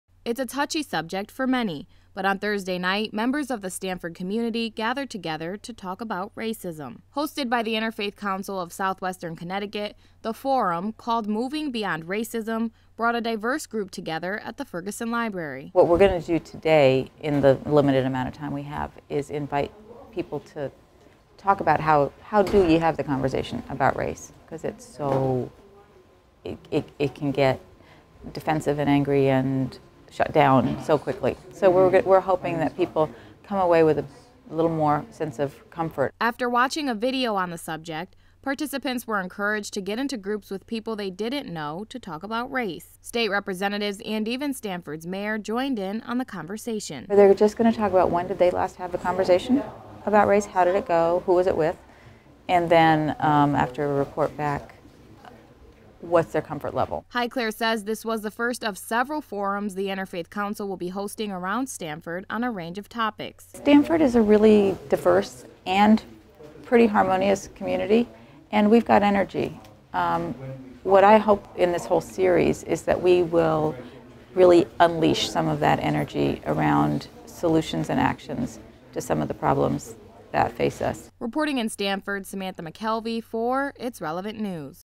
Interfaith Council hosts a conversation about race at Stamford's Ferguson Library
It's a touchy subject for many, but on Thursday night members of the Stamford community gathered to talk about racism. Hosted by the Interfaith Council of Southwestern CT, the forum, called Moving Beyond Racism, brought a diverse group together at the Ferguson Library.
After watching a video on the subject, participants were encouraged to get into groups with people they didn't know to talk about race. State representatives and even Stamford's Mayor joined in on the conversation. This was the first of several forums the Interfatih Council will be hosting around Stamford on a range of topics.